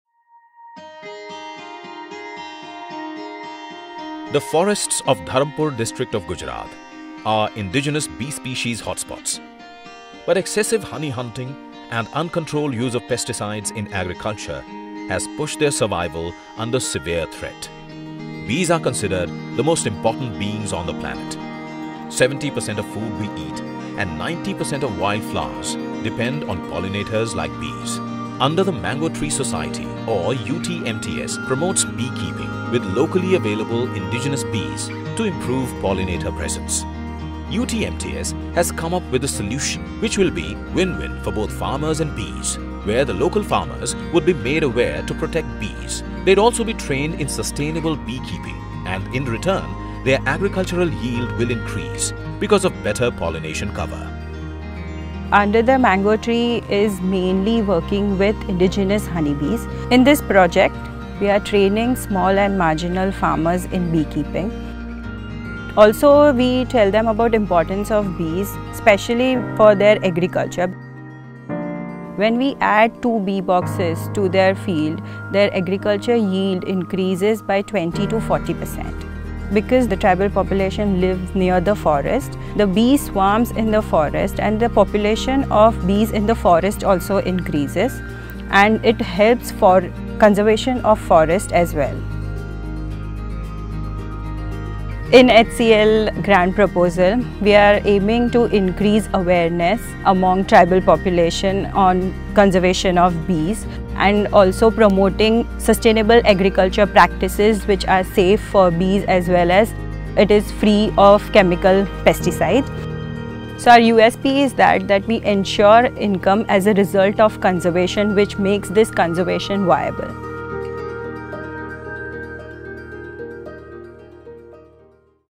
Under the Mango Tree (UTMT) - HCL Grant 2020 Recipient 2020, Environment Category
Increasing biodiversity through beekeeping! The HCL Grant Recipient in the Environment category @UTMT through the grant will be working on keeping Indigenous Bees Buzzing and Supporting Biodiversity Conservation in Tribal Communities. The project will impact smallholders in tribal communities and their neighbours in 115 villages in Gujarat, Madhya Pradesh and Maharashtra. UTMTS’s commitment lies in conservation and biodiversity, but it has developed a model which allows economic benefits to humans too—proving that conservation and human progress can go hand-in-hand.